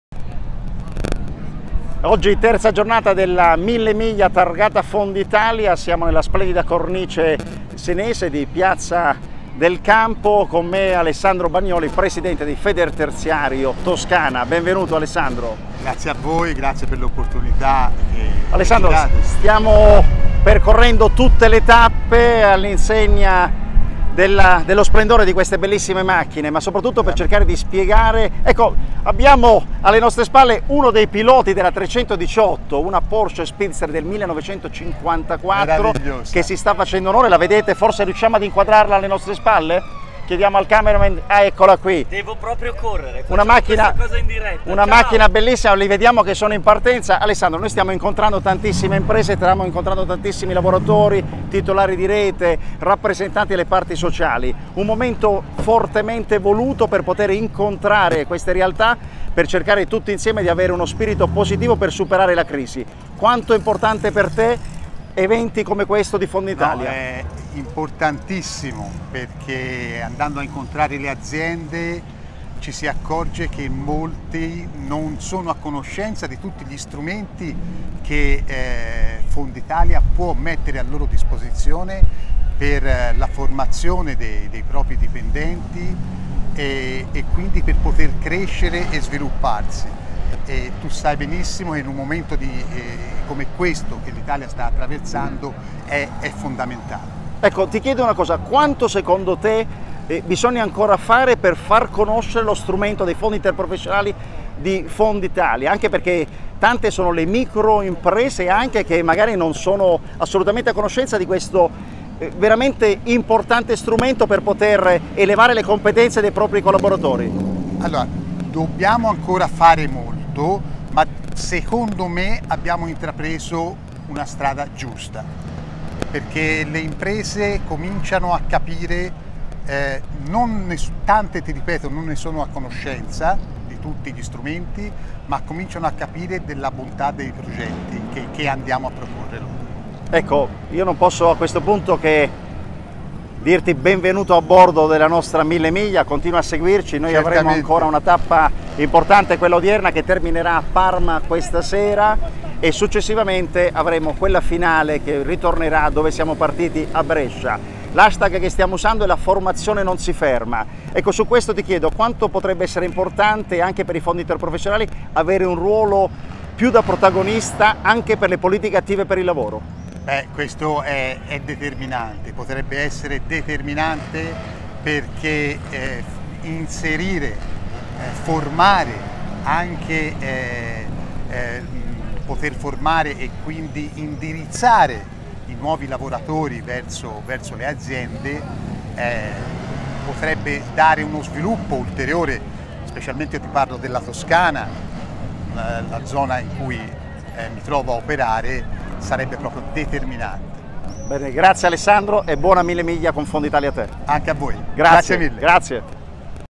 FondItalia alla 1000Miglia: intervista ad Alessandro Bagnoli Presidente Federterziario Toscana
FondItalia alla 1000Miglia: Massimo Maria Amorosini intervista Alessandro Bagnoli Presidente Federterziario Toscana. Imprese e lavoratori uniti per superare la crisi, molte aziende non sono a conoscenza degli strumenti che FondItalia mette a disposizione gratuitamente per la formazione. C’è ancora un lungo percorso da fare in questa direzione, ma siamo sulla strada giusta perché i numeri in crescita delle adesioni al Fondo lo dimostrano. In una fase di ripresa per il Paese, dopo una crisi lunga che ha penalizzato sviluppo e produttività, oggi dalla partecipazione dei Fondi anche alle politiche attive per il lavoro potrebbero arrivare i segnali giusti.